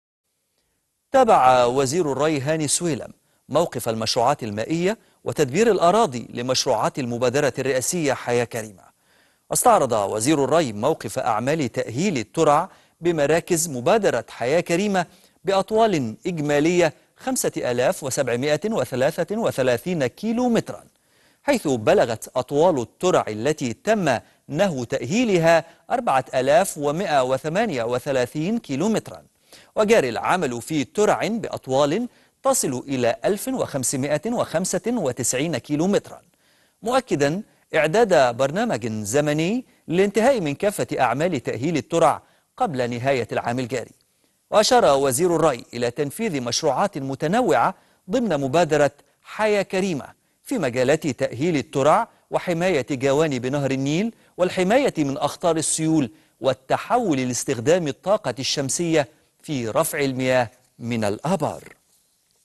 الأخبار - وزير الري يتابع موقف المشروعات المائية وتدبير الأراضي لمشروعات مبادرة "حياة كريمة"
نشرة الأخبار - وزير الري يتابع موقف المشروعات المائية وتدبير الأراضي لمشروعات المبادرة الرئاسية "حياة كريمة"